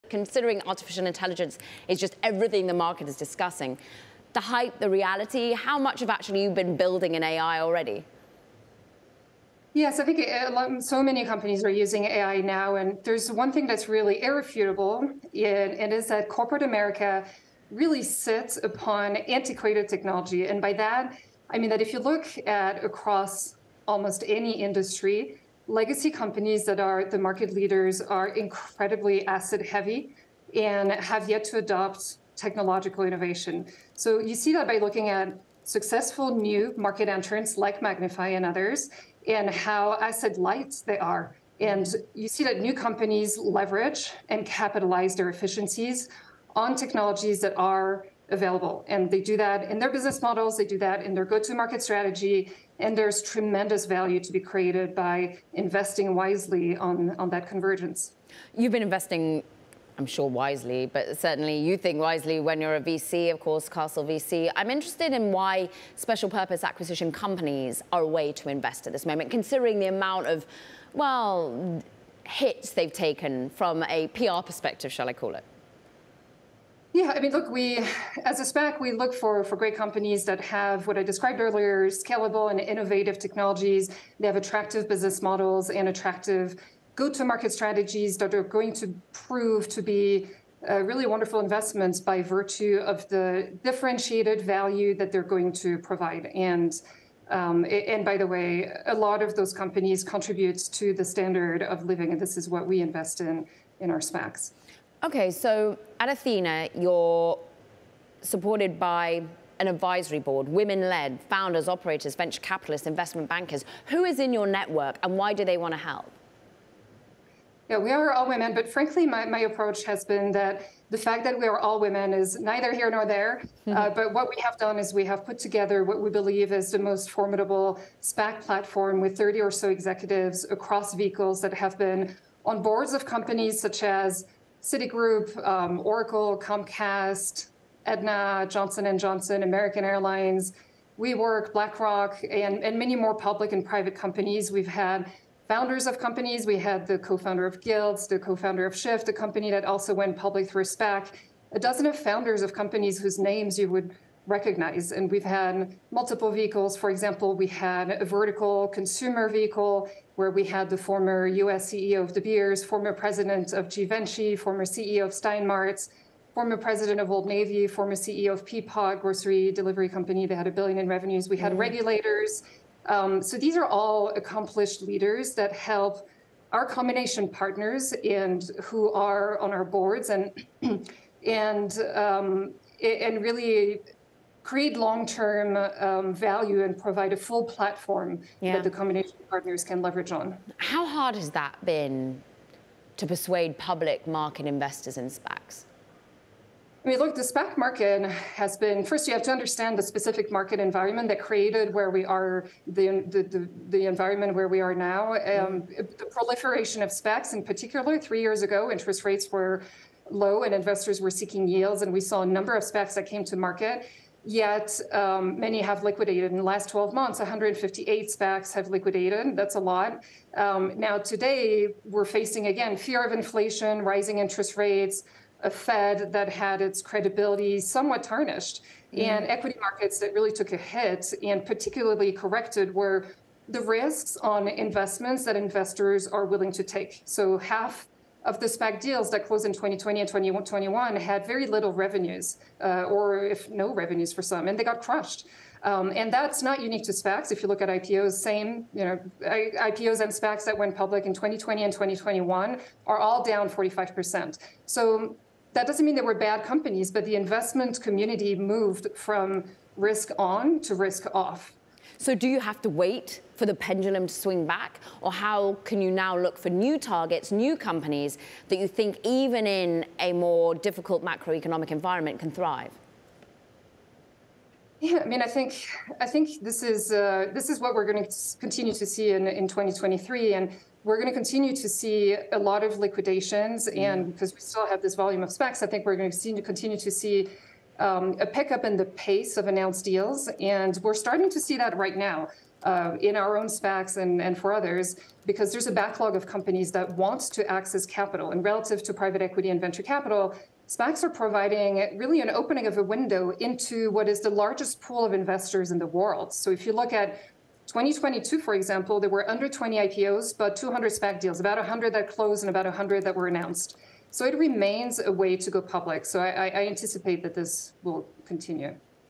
Athena Technology CEO on SPAC Outlook
Athena Technology CEO and Founder and Magnifi Co-Founder Isabelle Freidheim joins Caroline Hyde to discuss the rise of AI and her predictions for SPACs in 2023, as well as the potential opportunities and risks.